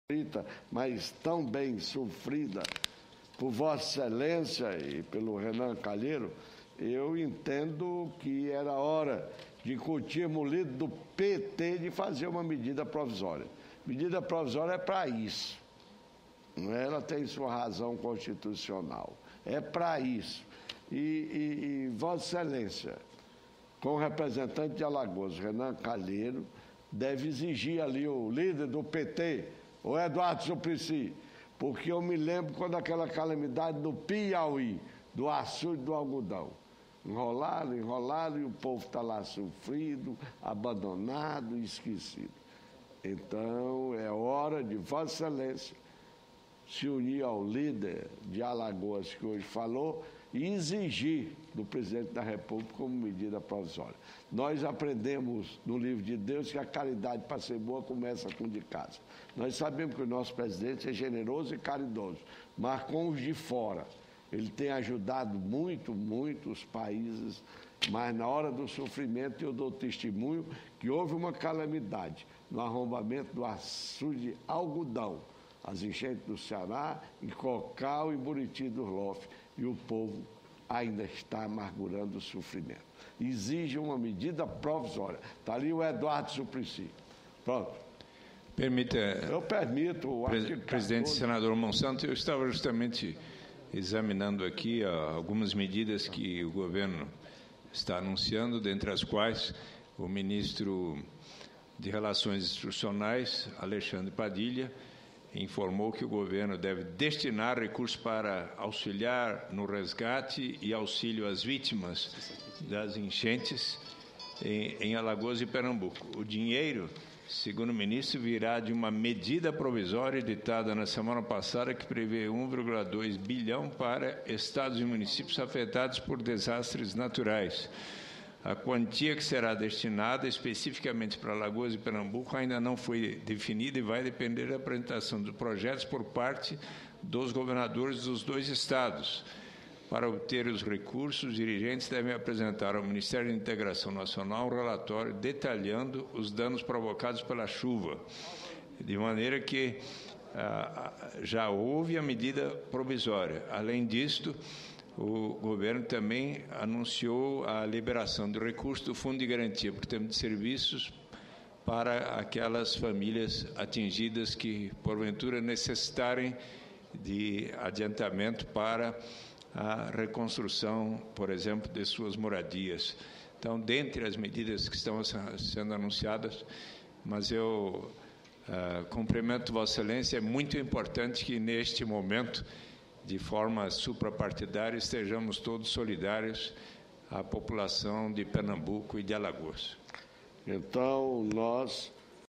Mão Santa e Suplicy debatem formas de socorro às vítimas das chuvas em Pernambuco, Alagoas e Paraíba
22/06/2010 - Plenário Discursos - Mão Santa e Suplicy debatem formas de socorro às vítimas das chuvas em Pernambuco, Alagoas e Paraíba